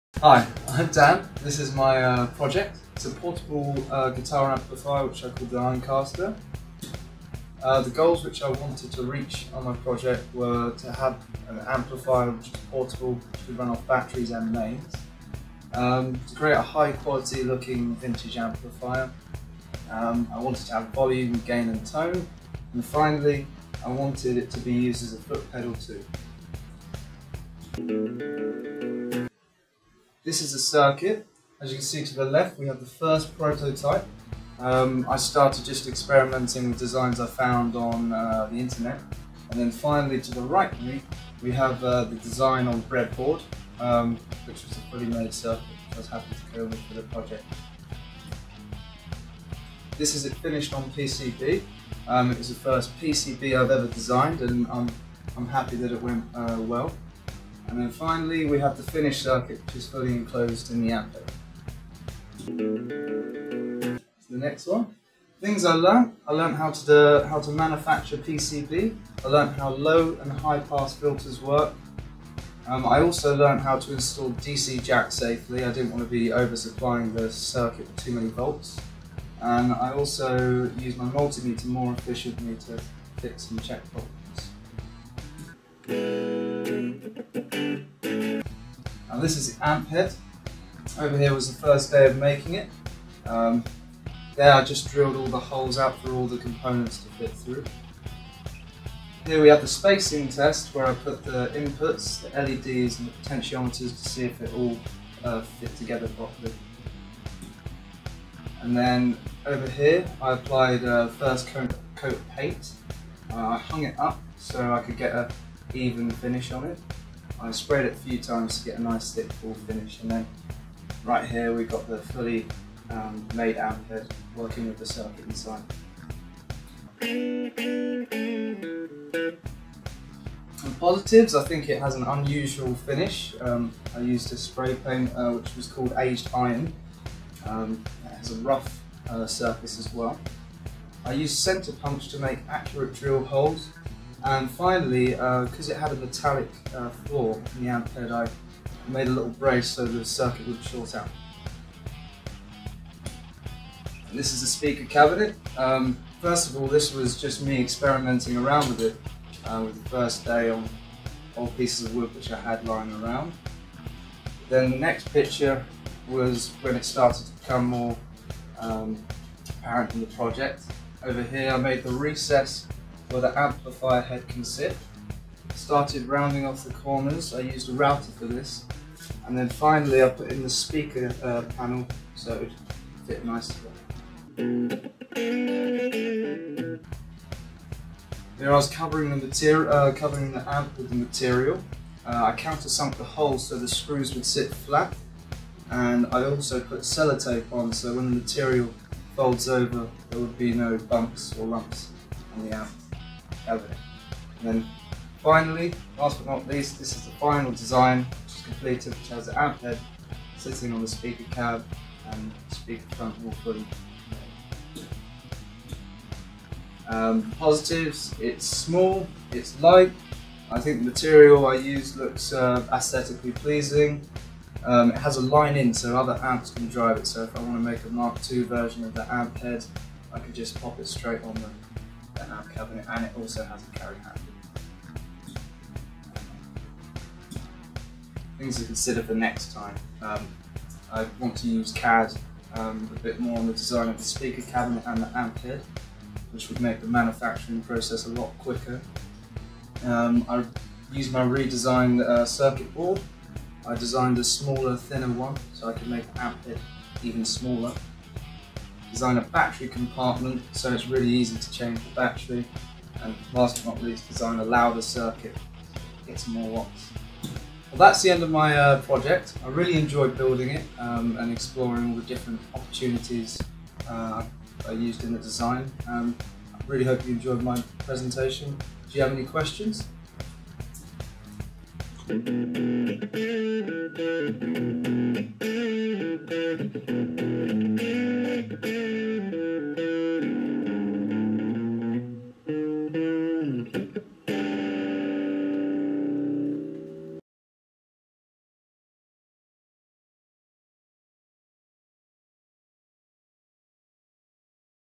Perfect BTEC Project Presentation
This is an excellent example of a BTEC engineering project presentation given at level 3.